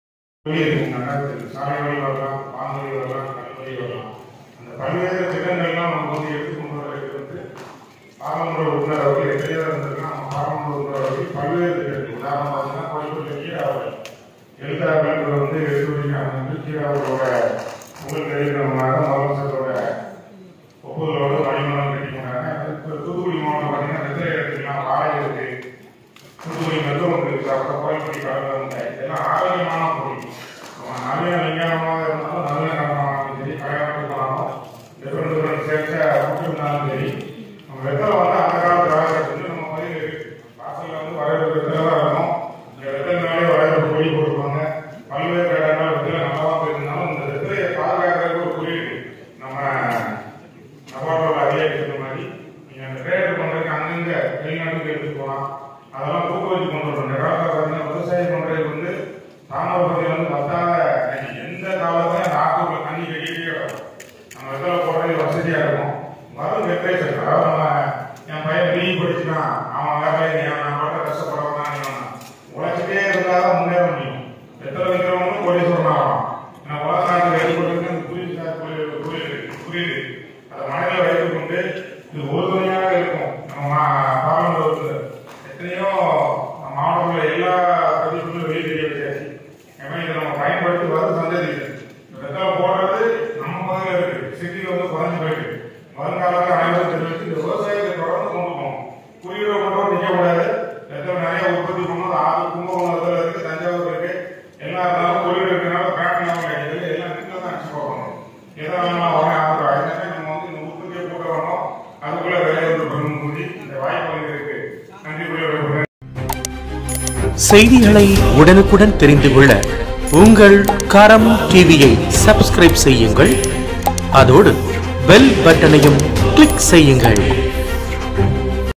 வெற்றிலைக்கு புவிசார் குறியீடு மேயர் ஜெகன் பெரியசாமி பாராட்டு
karamtv உலக புகழ்பெற்ற ஆத்தூர் வெற்றிலைக்கு புவிசார் குறியீடு தூத்துக்குடி ஆட்சியர் அலுவலகத்தில் நடைபெற்ற விழாவில் வெற்றிலை விவசாயிகளிடம் கனிமொழி கருணாநிதி எம்பி வழங்கினார். இந்த நிகழ்ச்சியில் மேயர் ஜெகன் பெரியசாமி பேசினார்